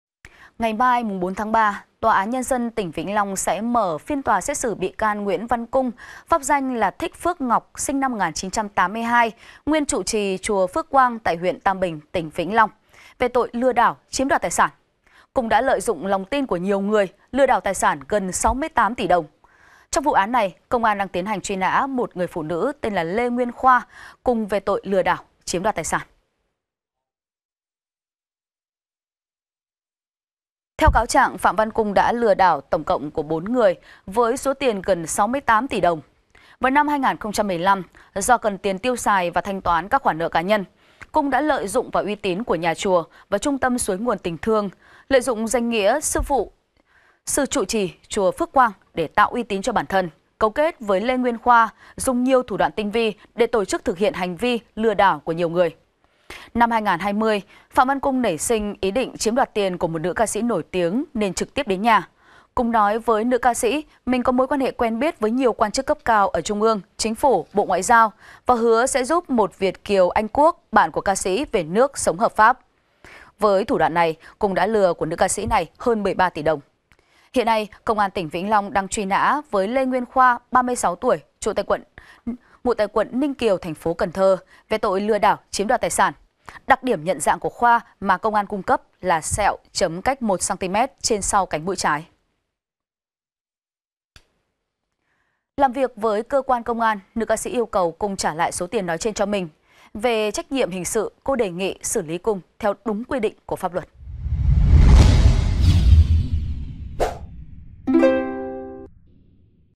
Vụ cựu Trụ trì chùa lừa đảo gần 68 tỷ đồng, truy nã 1 phụ nữ | VTC14
VTC14 |  VỤ CỰU TRỤ TRÌ CHÙA LỪA ĐẢO GẦN 68 TỶ ĐỒNG, TRUY NÃ 1 PHỤ NỮ
Ngày mai (4/3), TAND tỉnh Vĩnh Long sẽ mở phiên tòa xét xử bị can Nguyễn Văn Cung (pháp danh Thích Phước Ngọc, sinh năm 1982, nguyên trụ trì chùa Phước Quang (huyện Tam Bình, Vĩnh Long) về tội “Lừa đảo chiếm đoạt tài sản”. Cung đã lợi dụng lòng tin của nhiều người, lừa đảo tài sản gần 68 tỉ đồng. Trong vụ án này, Công an đang tiến hành truy nã một người phụ nữ tên Lê Nguyên Khoa cùng về tội lừa đảo chiếm đoạt tài sản.